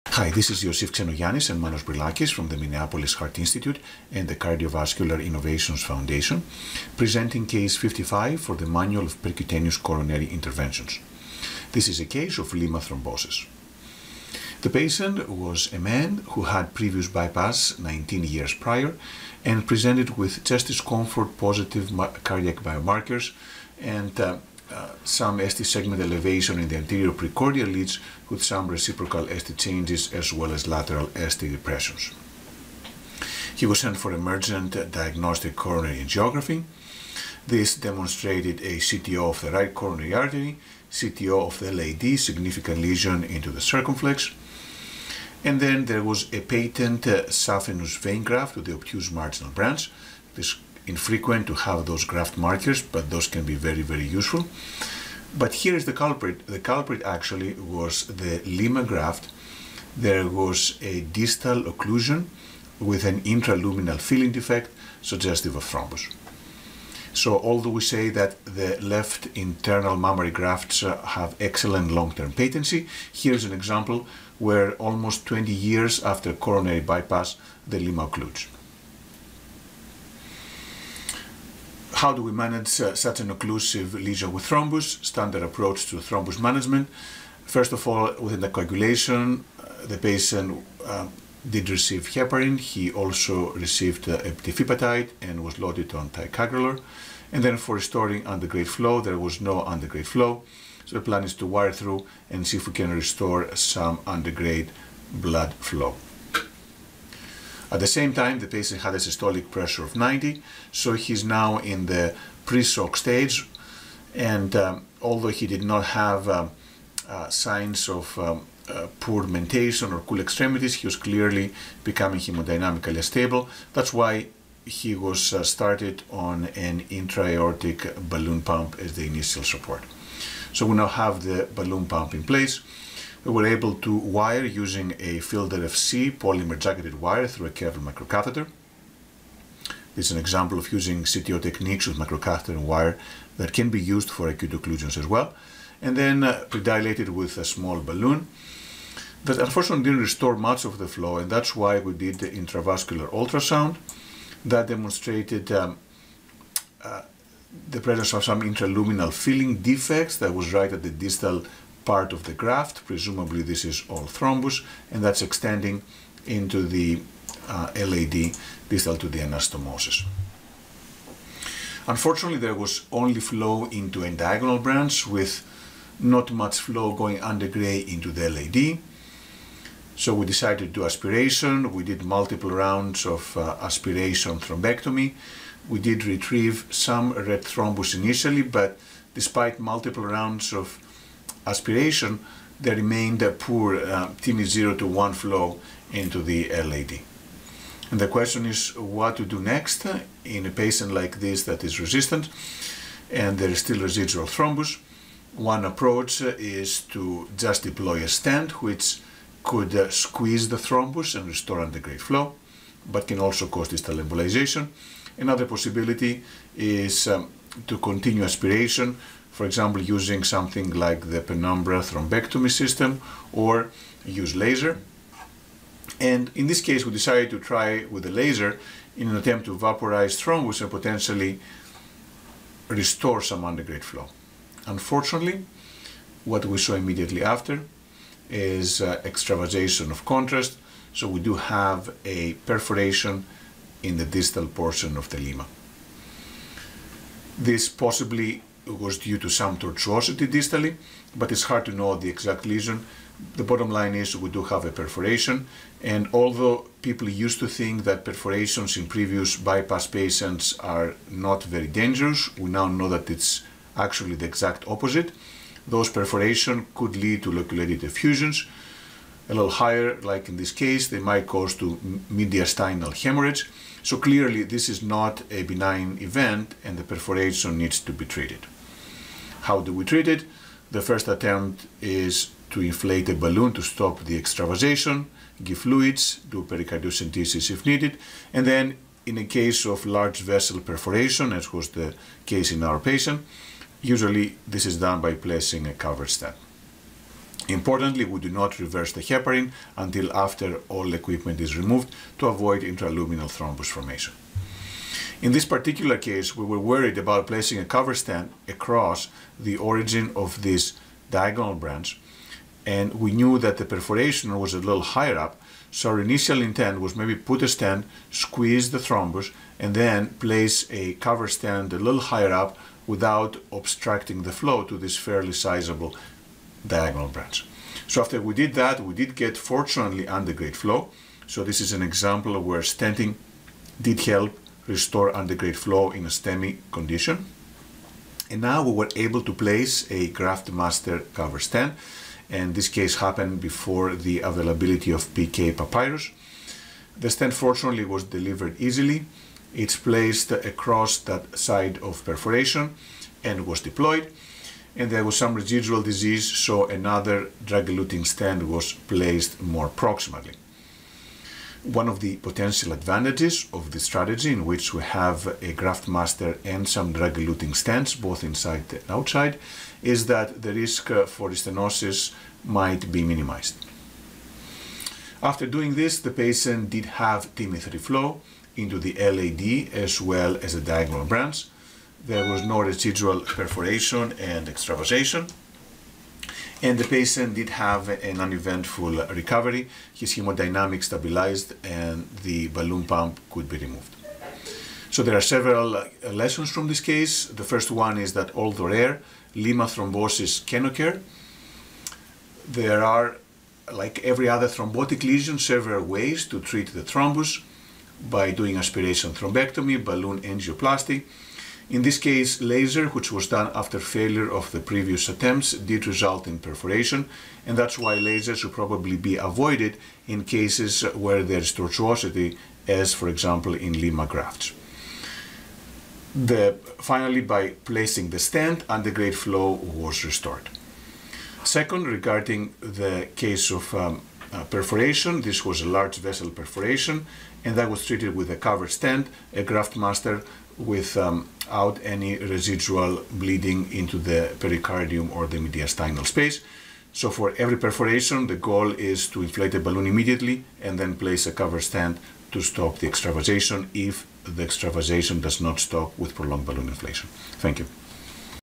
Case 55: PCI Manual - LIMA thrombosis
A patient presented with acute myocardial infarction due to thrombosis of the distal LIMA anastomosis. He was also hypotensive with systolic blood pressure of 90 mmHg. The thrombosed graft was wired with a Fielder FC wire, however balloon angioplasty and aspiration thrombectomy failed to restore antegrade flow. Laser was performed, but led to distal LIMA perforation. A 2.75x30 mm DES was placed from the LIMA into the LAD restoring TIMI 3 antegrade flow, followed by placement of a Graftmaster stent across the perforation segment, achieving hemostasis. The patient's hemodynamics stabilized and he had an uneventful recovery.